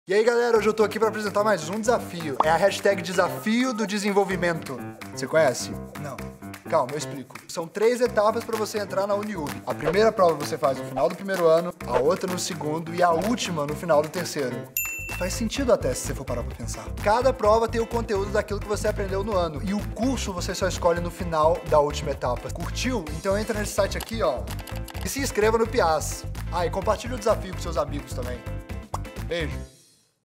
Pias 2017 UNIUBE
Vídeo produzido para WEB e TV com o Youtuber Robert Kifer para a Universidade UNIUBE

Produto: WEB/TV
Produtora de Vídeo: Vagalume Filmes
Agência: Futura P Comunicação
Cliente: UNIUBE
Direção: Mauro Pianta
Direção de Fotografia: Mauro Pianta
Edição/Motion/Finalização: Christiano Pires
Produção/Direção de Arte: Cezar Campos
Youtuber: Robert Kifer